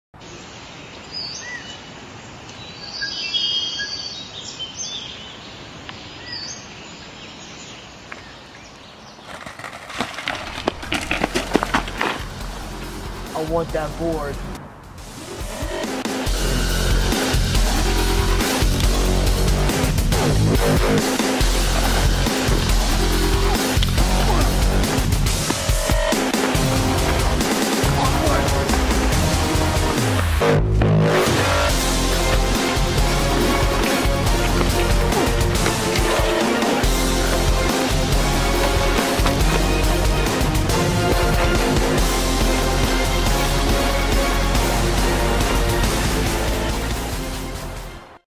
The Almighty Skateboard
Just a random skit with Shawn and James, filmed and edited by Nick, enjoy :D